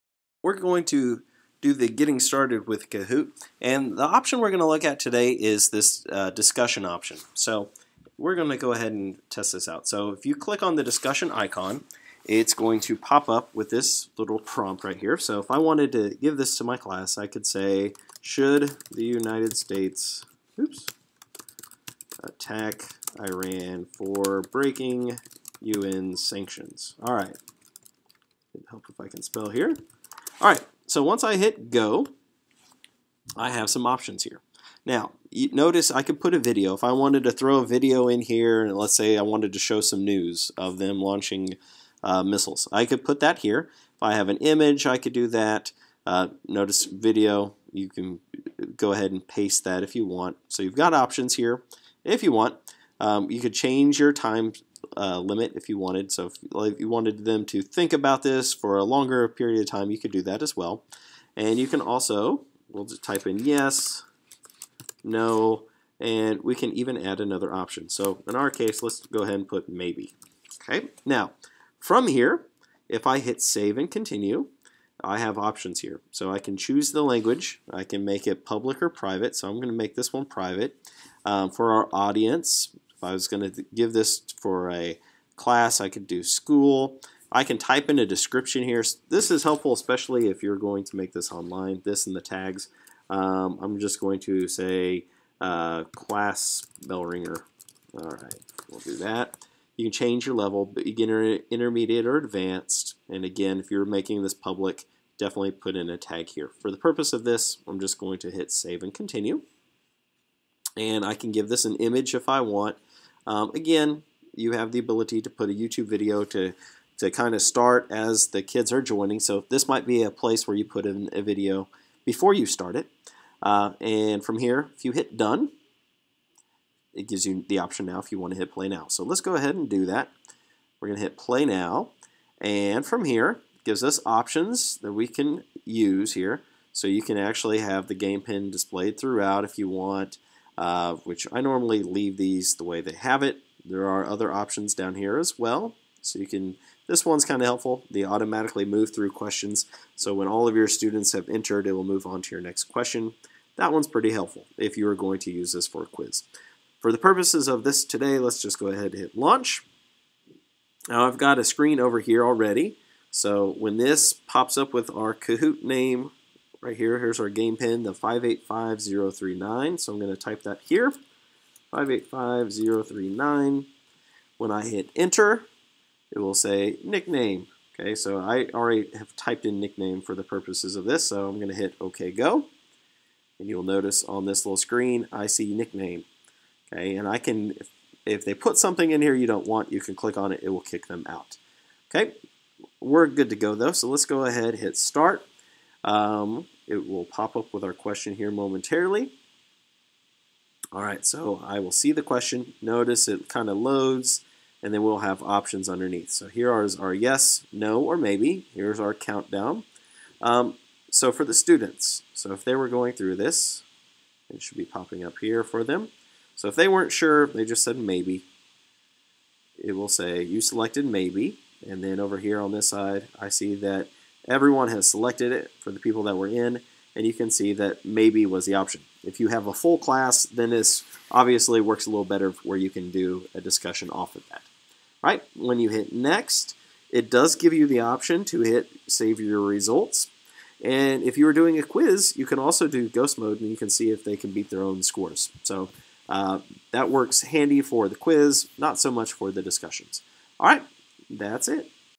Getting Started with Kahoot Video 3 Discussions
This video goes through the Discussions option in Kahoot.